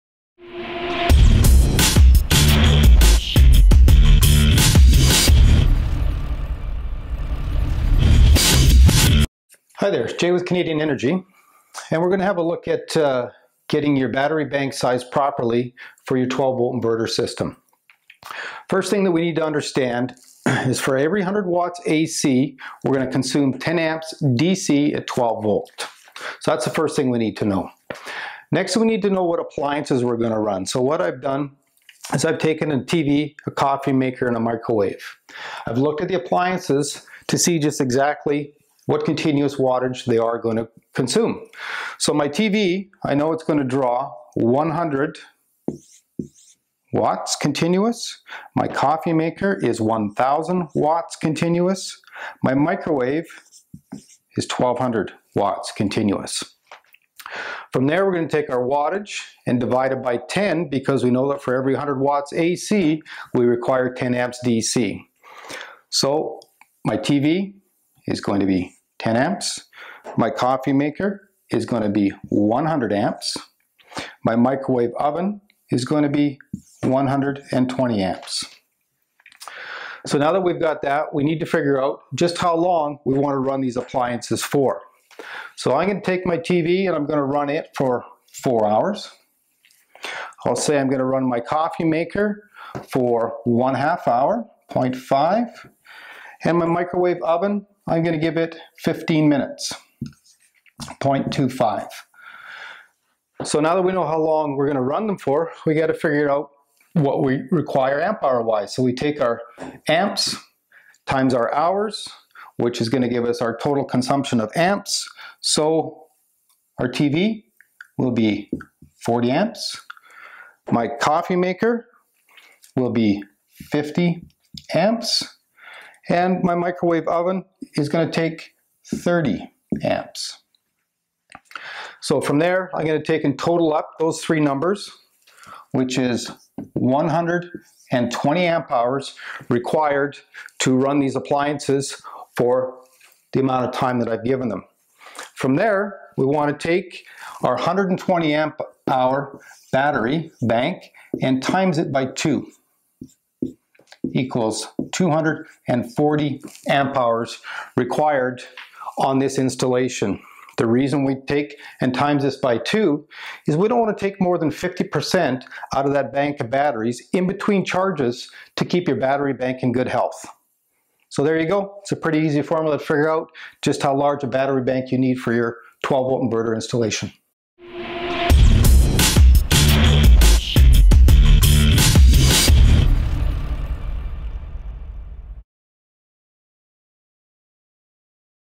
Canadian Energy™: Sizing a Battery Bank
Jay walks us through sizing a battery bank.